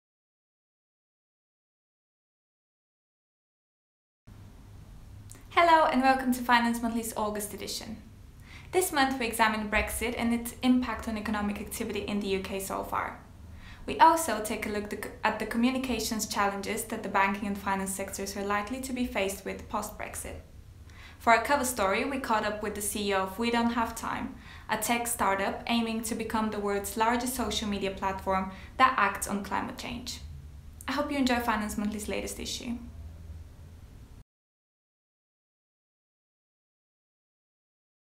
Editors' Picks August 2018
Here we have this months editor's picks!

In this months edition we look at the communications challenges that the banking and finance industry are likely to be faced with post-Brexit.